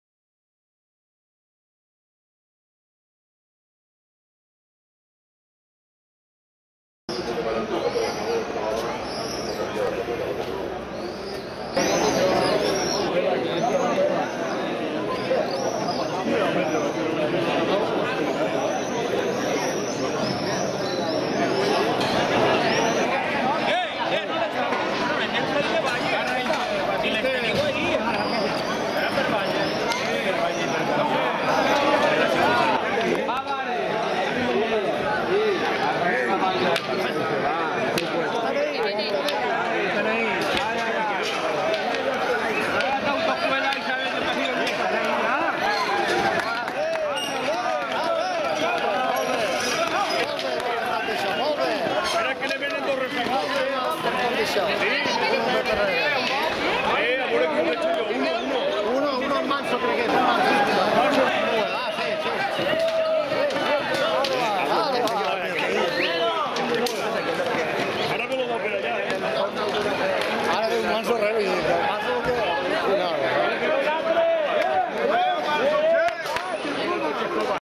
Encierro San Juan 09
Encierro con toros de El Saliner el 23 de junio de 2009 en Puçol (Valencia).